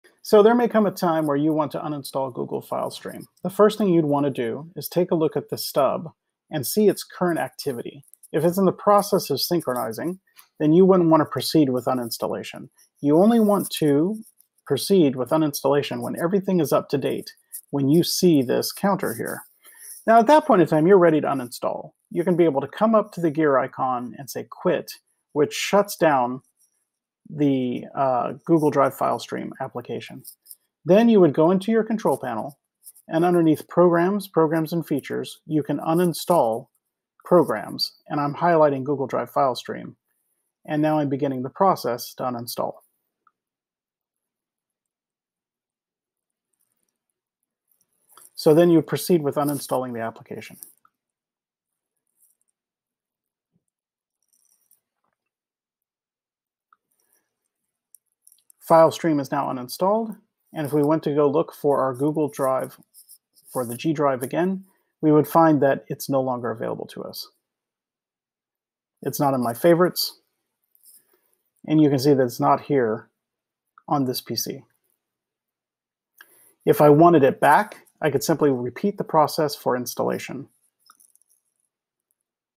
How to Uninstall Google Drive File Stream
Here's some quick instructions on how to uninstall Google Drive File Stream from a Windows PC.